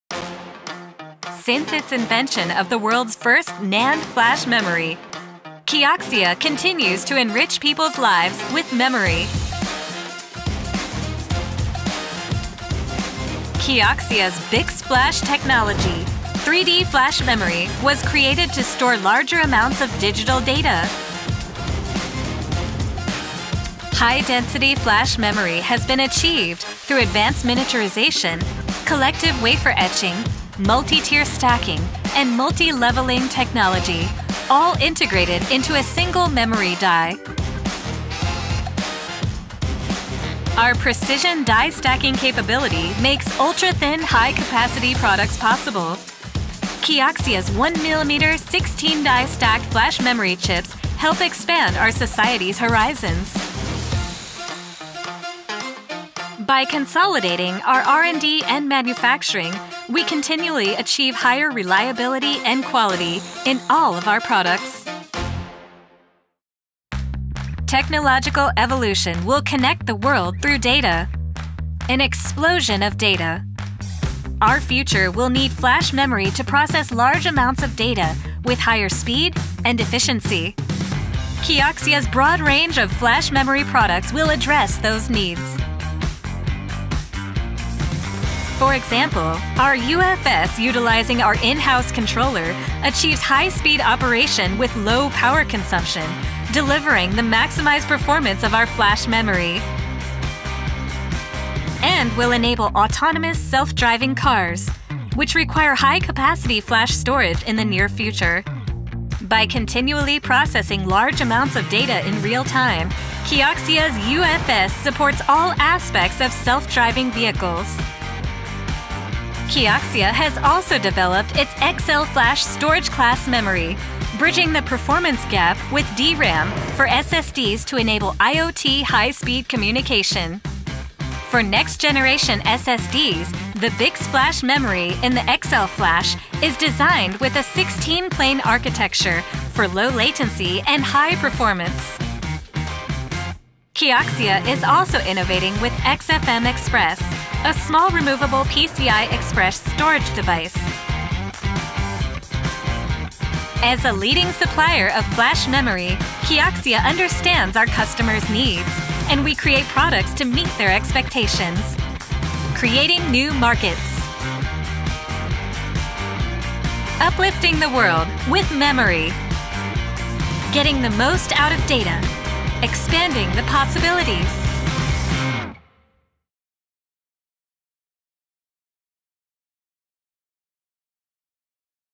KIOXIA - Memory Products introduction movie - (Extended Version)
Introduction to Kioxia flash memory products and technologies.
Kioxia innovates with its BiCS FLASH™ 3D memory and wide variety of products to address a range of customer needs and applications.